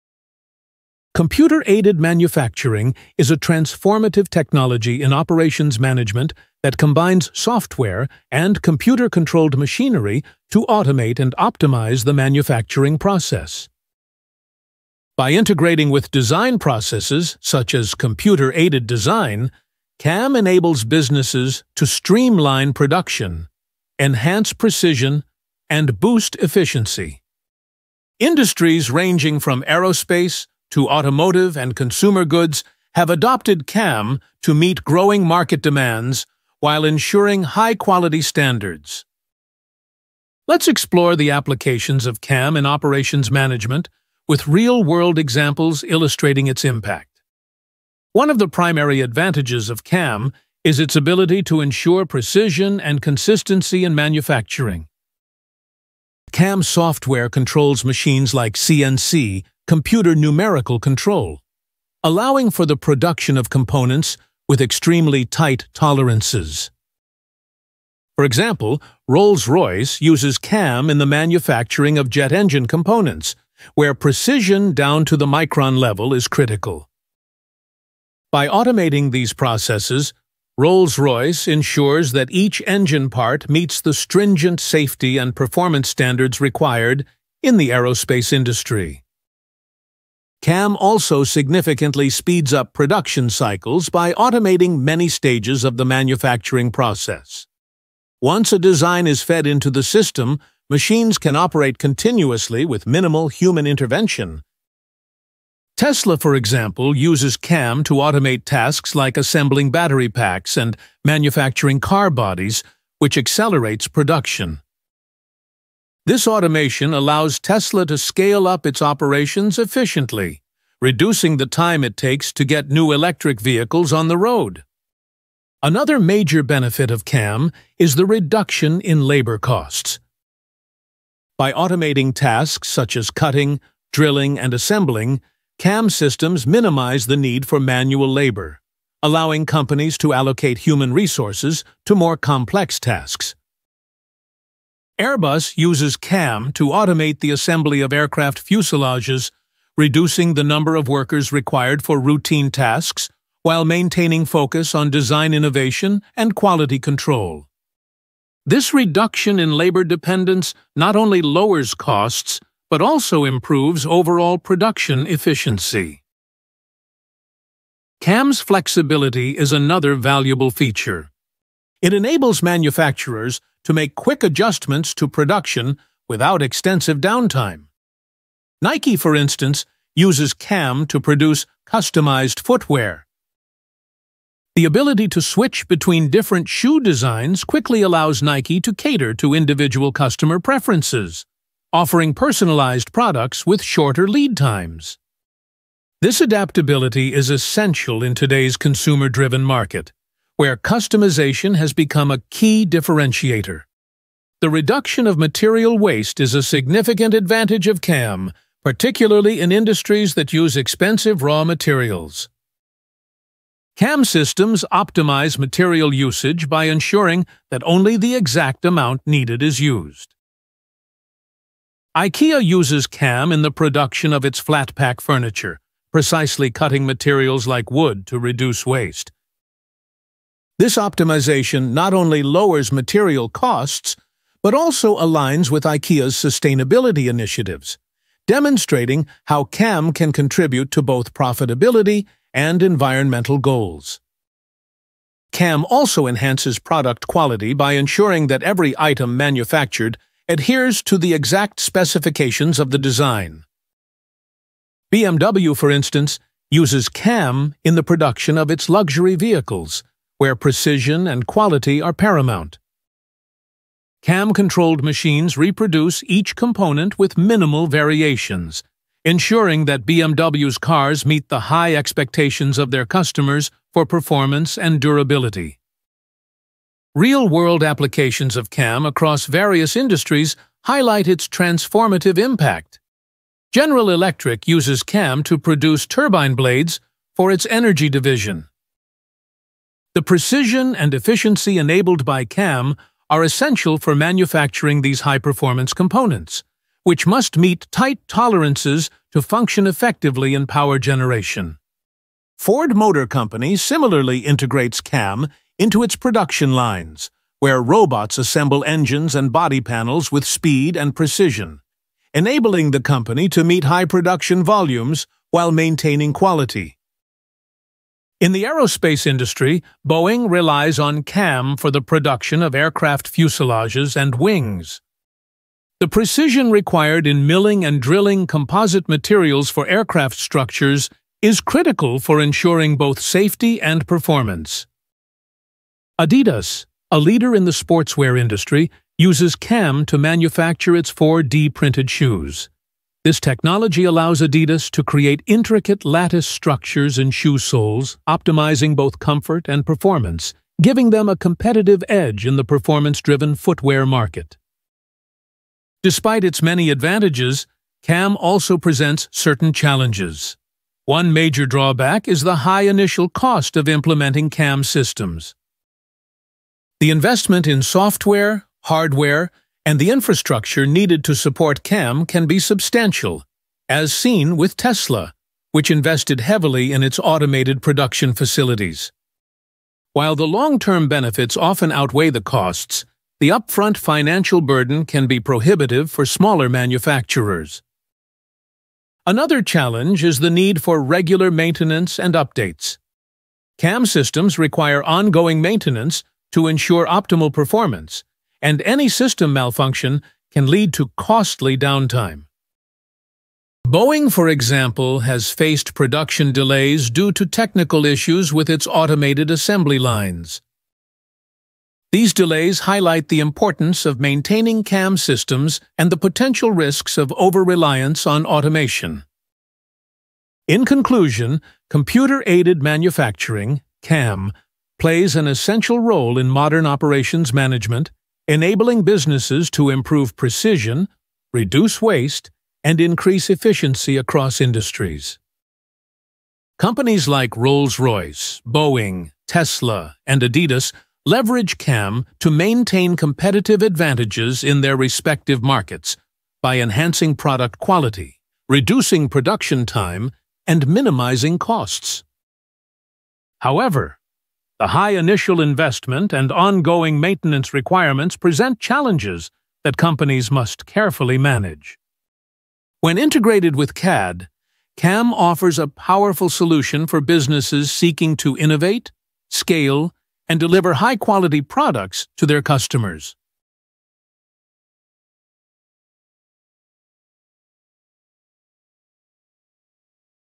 How Computer-Aided Manufacturing (CAM) Revolutionizes Operations?
Explore the transformative power of Computer-Aided Manufacturing (CAM) in modern operations management! This video details how CAM integrates software and computer-controlled machinery to automate production, boost efficiency, and ensure precision. We'll dive into real-world applications across various industries, from aerospace (Rolls-Royce, Boeing) to automotive (Tesla, Ford, BMW) and consumer goods (Nike, IKEA, Adidas, GE).

Perfect for VCE Business students and anyone keen to understand how CAM enhances product quality, reduces waste, cuts labor costs, and improves flexibility in today's competitive market. We also touch upon the challenges of high initial investment and ongoing maintenance.

I hope the video takes you beyond the definition & helps to make the learning process a little more tangible.

Video Content

0:00 - Introduction to Computer-Aided Manufacturing (CAM)
0:40 - Applications and Advantages of CAM
0:48 - Precision and Consistency (Rolls-Royce Example)
1:30 - Increased Production Speed (Tesla Example)
2:05 - Reduced Labor Costs (Airbus Example)
2:46 - Flexibility (Nike Example)
3:23 - Reduction of Material Waste (IKEA Example)
4:04 - Enhanced Product Quality (BMW Example)
4:37 - Real-World CAM Applications Across Industries (GE, Ford, Boeing, Adidas)
5:56 - Challenges of CAM Implementation
6:00 - High Initial Investment (Tesla Example)
6:28 - Maintenance and Technical Support (GE, Boeing Examples)
7:00 - Conclusion: The Role of CAM in Modern Operations

Famous Quote: "Quality is never an accident; it is always the result of intelligent effort." — John Ruskin

School of Learning
Arnav Dutta